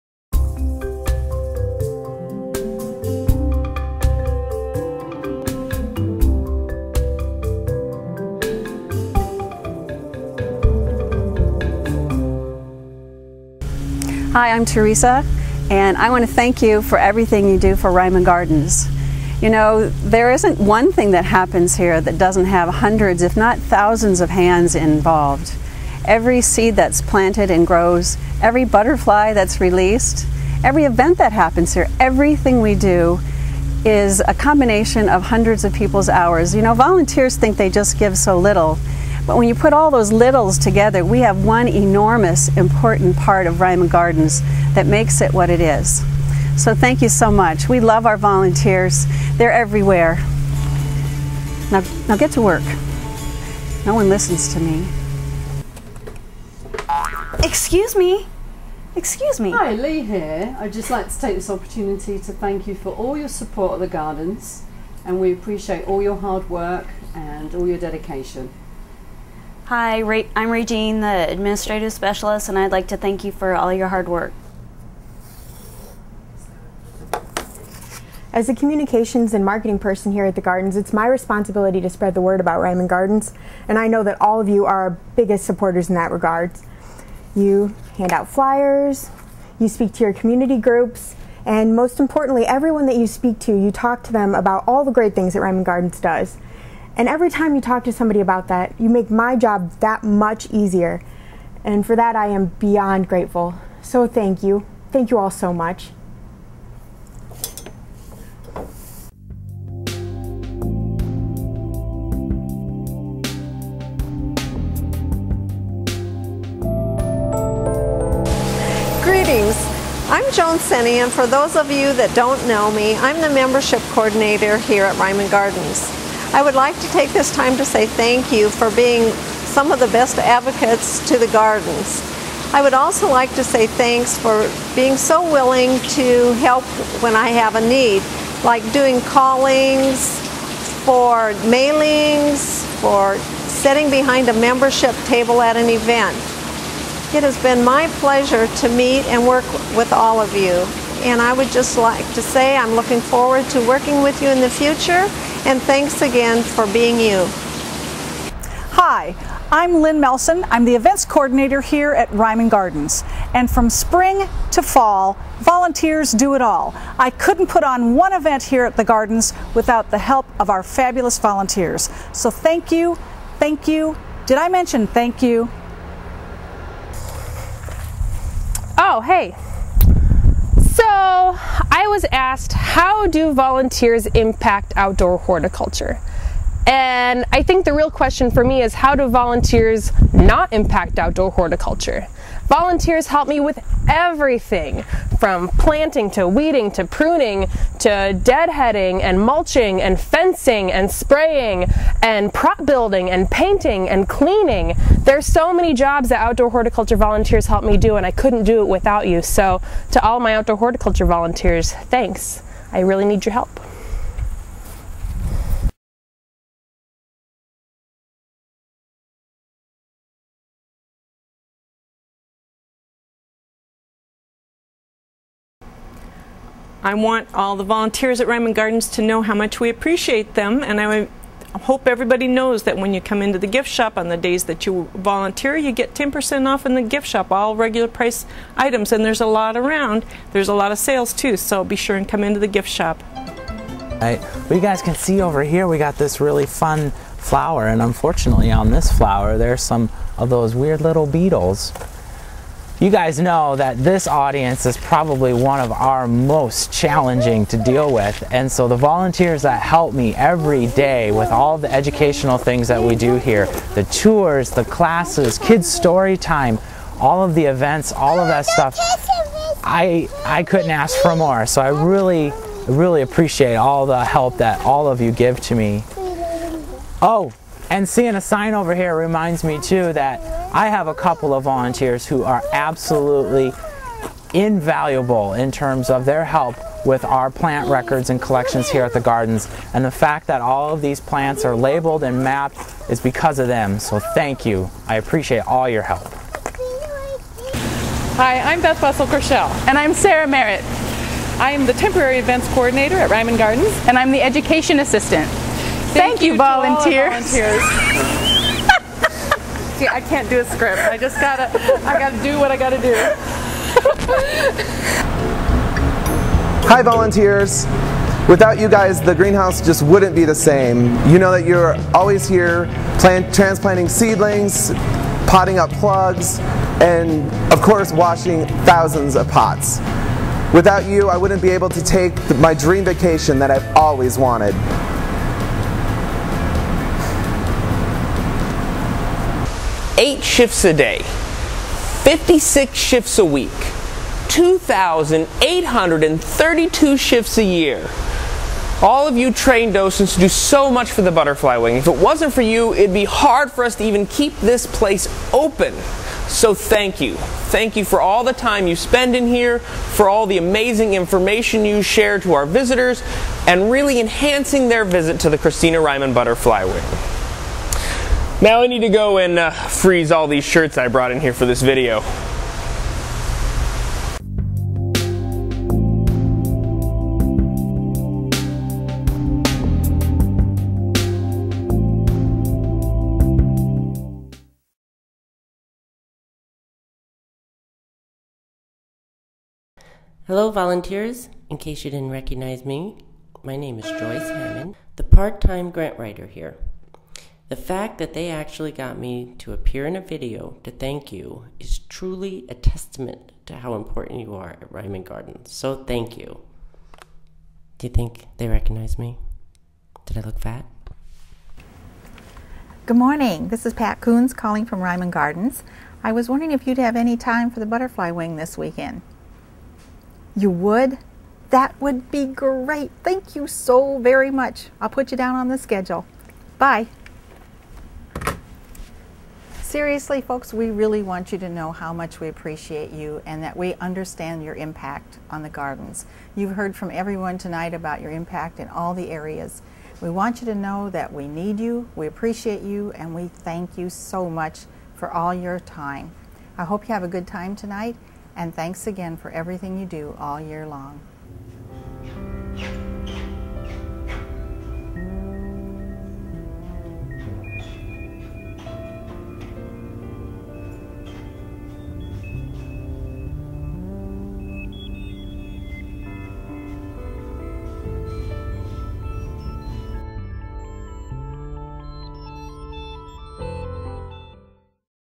2011 Reiman Gardens Volunteer Appreciation.wmv
Video that was presented at the 2011 Reiman Gardens fall volunteer appreciation event to express the staffs appreciation for all their hard work.